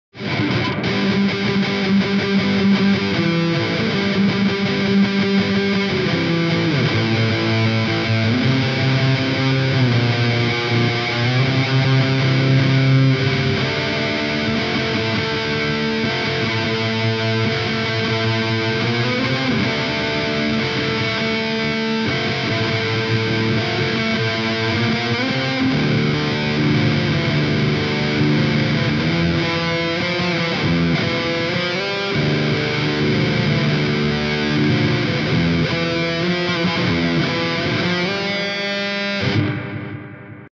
Who know this track?
Some guitar piece from Sweden melodic death metal band! Sound Solitude Amplitube 4! If you know this track write your answer in comments!

A lot of instrumental guitar ballads you can listen in Telegram